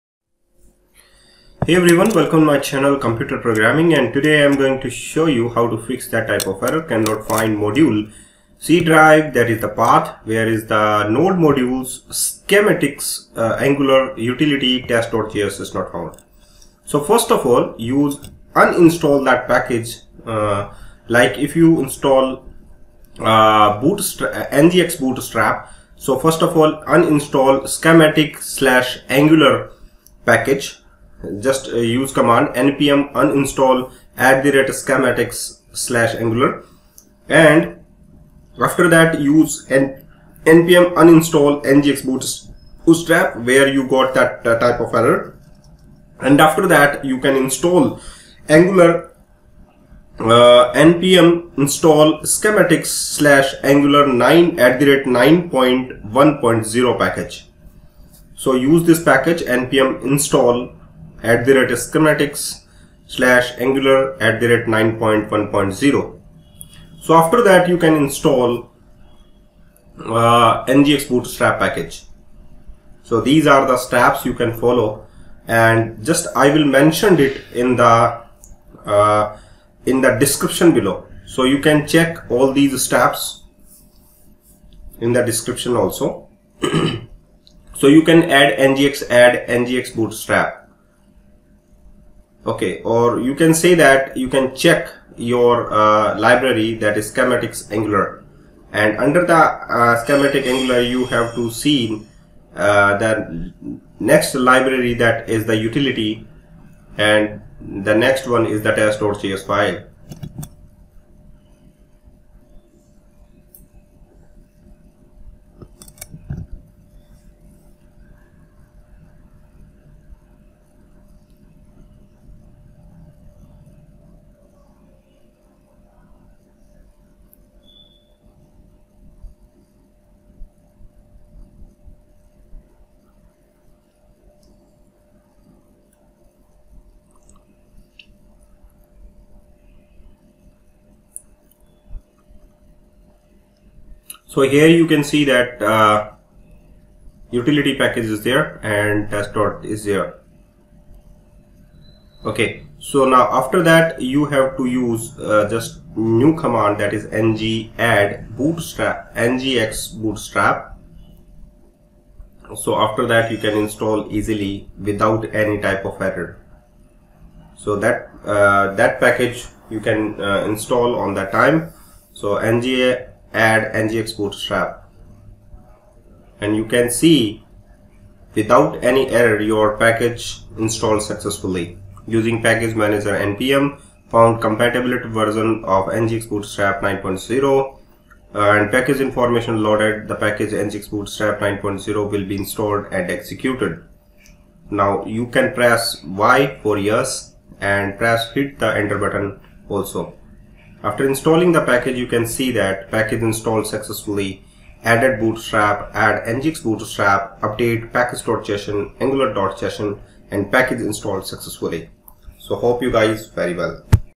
Fix: Cannot find module nodemodule/@schematics/angular/utility/test.js
Fix:  Cannot find module \nodemodules\@schematics\angular\utility\test.js
Use these steps to resolve the problem
1. Uninstall package that you want to install like
npm uninstall ngx-bootstrap
2. uninstall @schematics angular package
npm uninstall @schematics/angular
3. Install @schematics Angular 9.1.0
npm install @schematics/angular@9.1.0
4. Install your package
ng add ngx-bootstrap